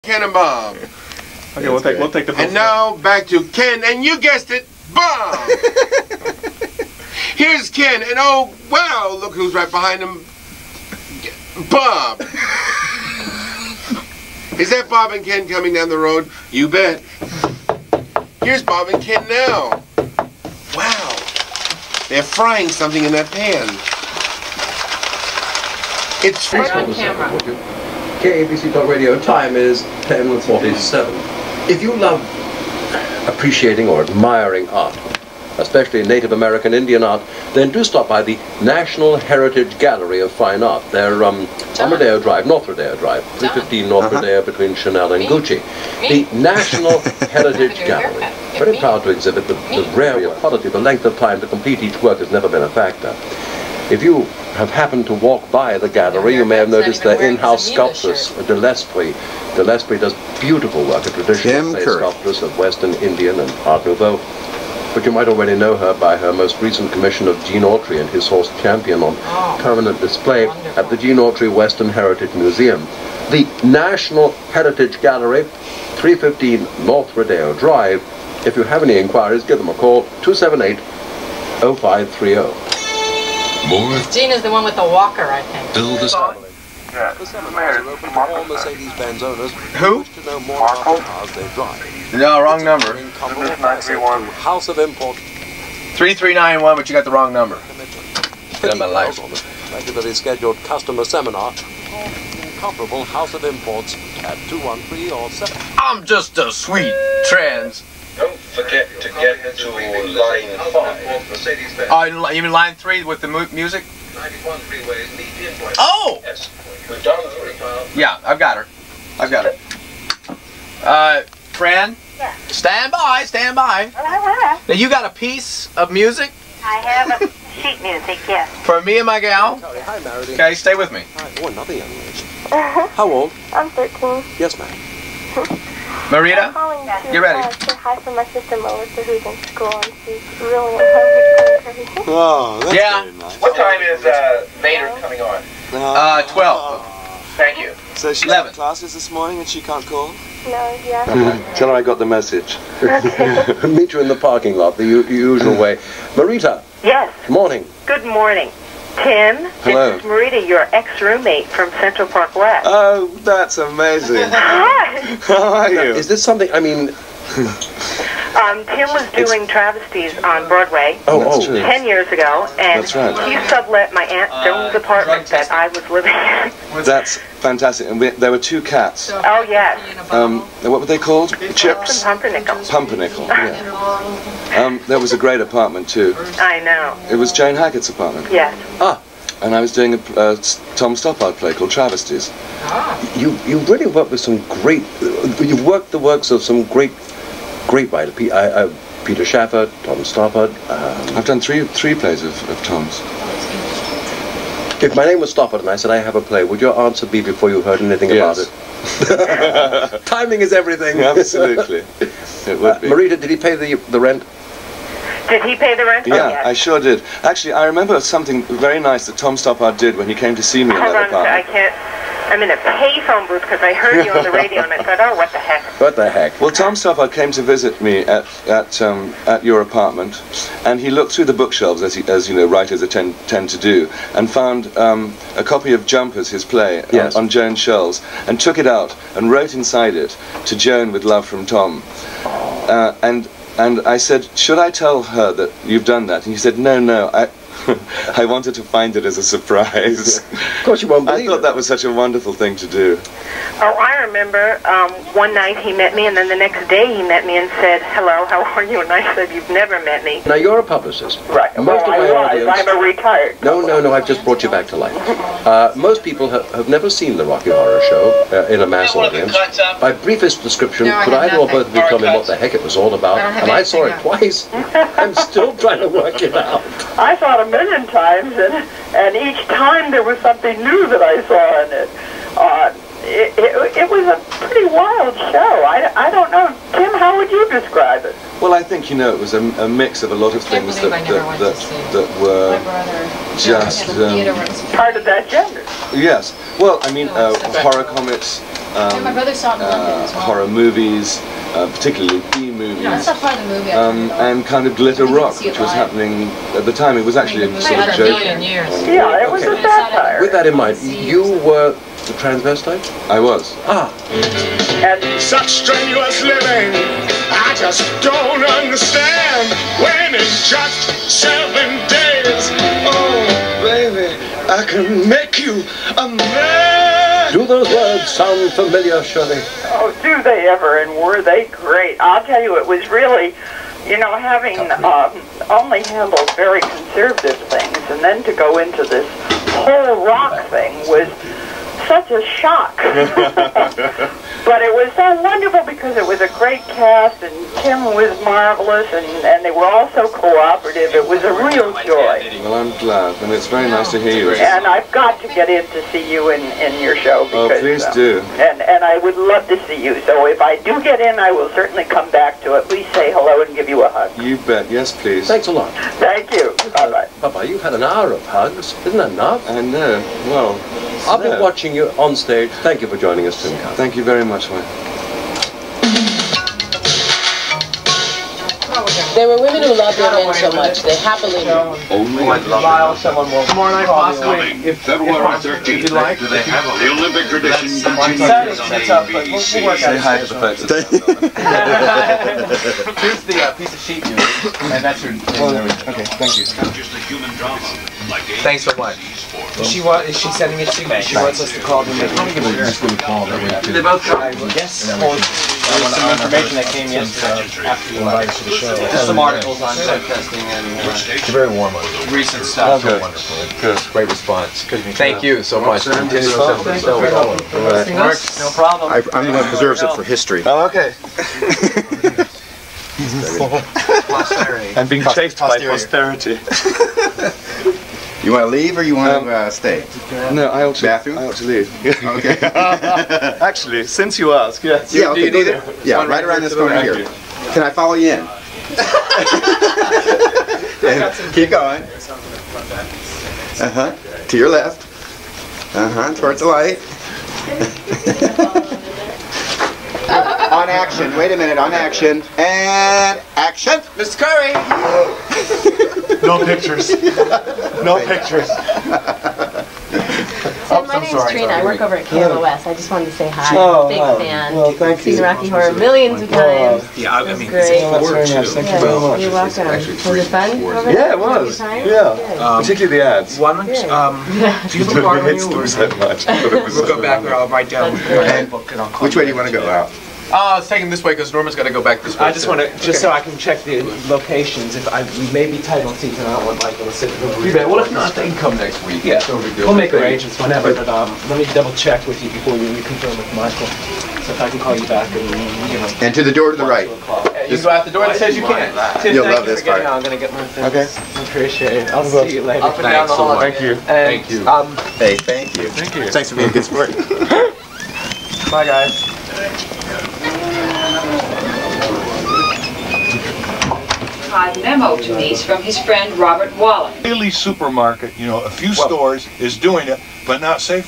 Behind the scenes. Part 3. Tim Curry
Babyboy is looking delish! ❤️

"This, the third in a series of five documentaries showing "behind-the-scenes" glimpses of celebrities being interviewed by KABC Talkradio host Michael Jackson,  feature the following personalities in the order of their appearance: Martin Sheen, Valerie Harper, Art Linkletter, Milton Berle, Leonard Maltin, Dom DeLuise, Tim Curry, Ralph Nader, Bette Davis, Helen Hayes and Robert Wagner."